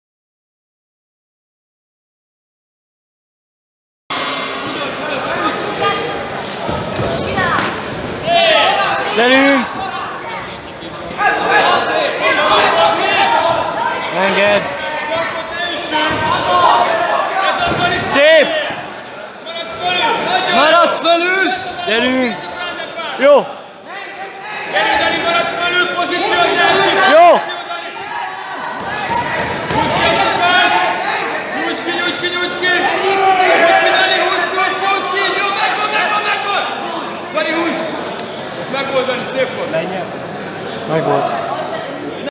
Kótai Dániel (Militans MMA) - Win Via Armbar At Kinizsi Kupa 2019
Kótai Dániel győzelme karfeszítéssel egy nála 8 kilóval nehezebb ellenféllel szemben, K-3 szabályrendszerben. / Dani's win via armbar under K-3 rules at the Kinizsi Kupa 2019 event.